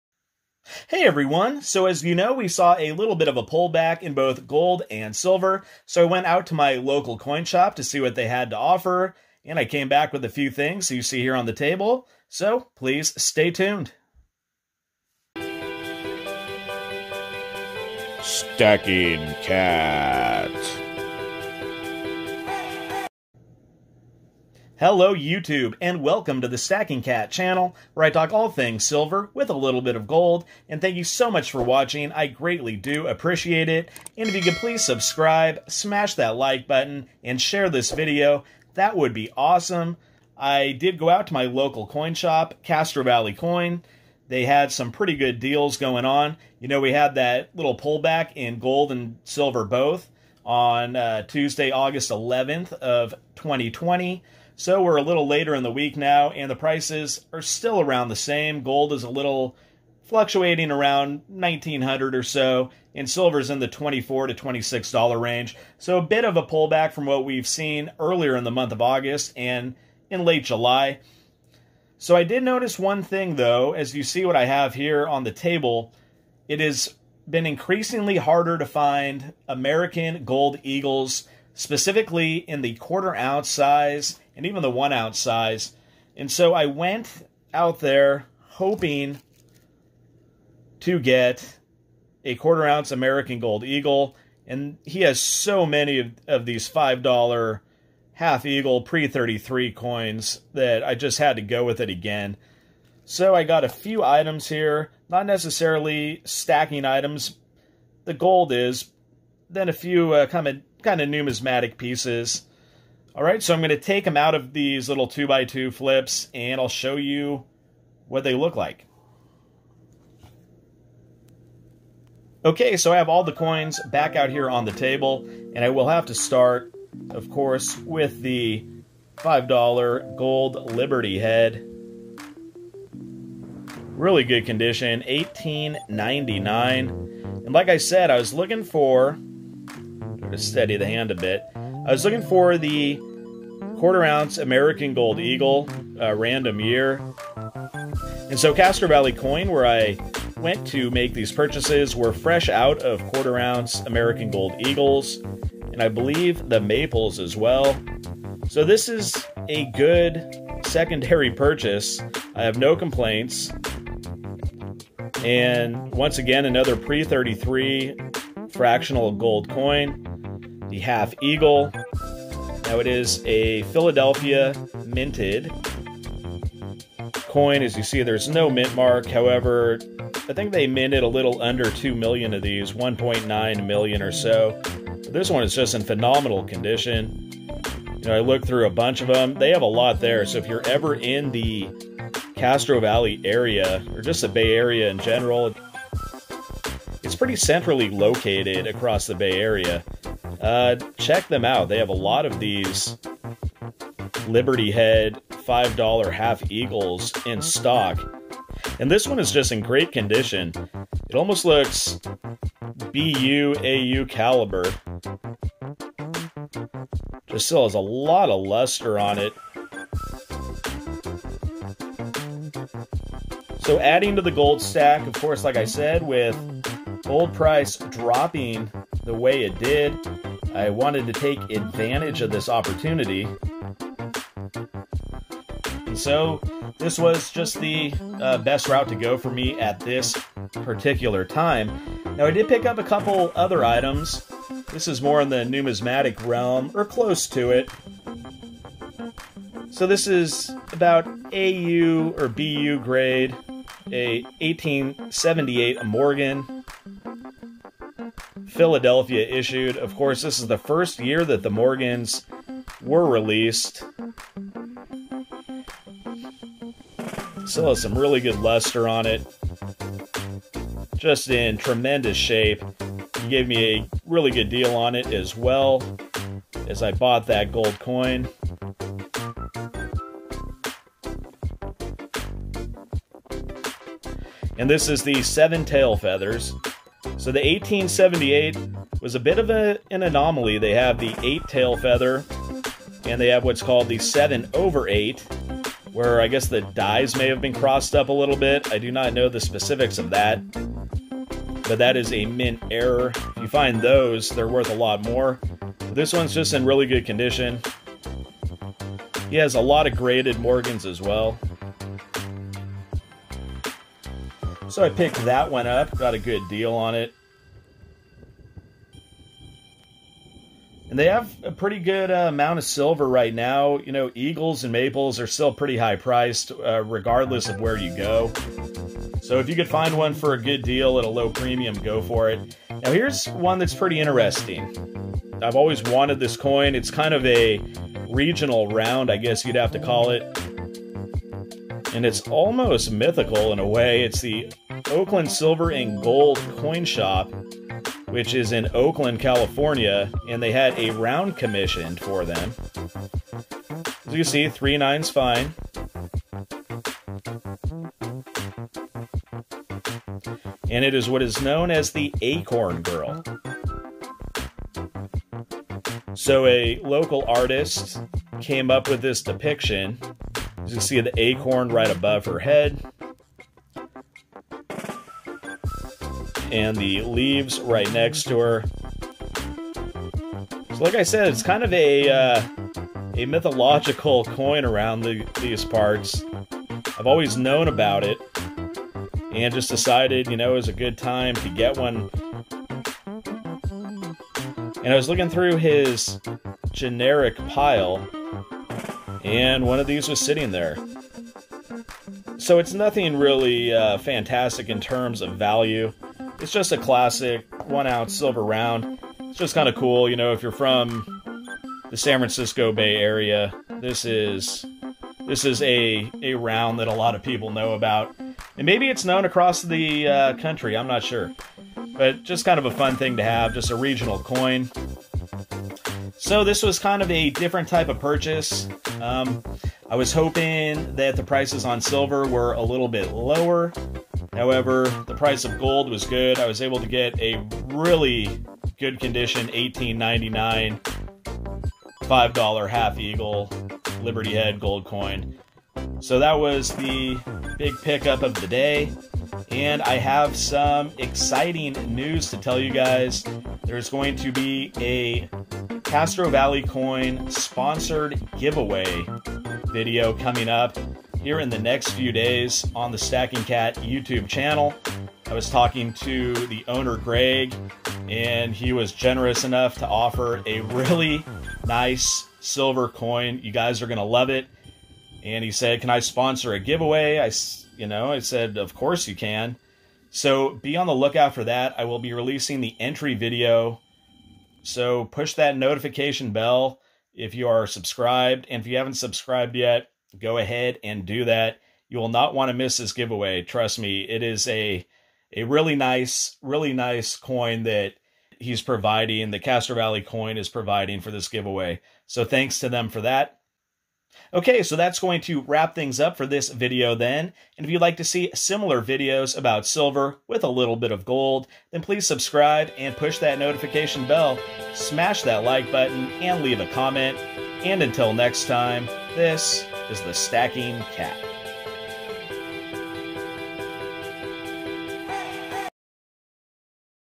Silver & Gold | New LCS Buys
Please tune-in as I head out to Castro Valley Coin for some silver and gold. At the time of this video gold had dropped back down to the $1,900+ range.

Castro Valley Coin:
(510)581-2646

*I am not a financial advisor. This channel and its videos are for the sole purpose of entertainment. The Stacking Kat channel is not responsible for the decisions made by individuals who view its content.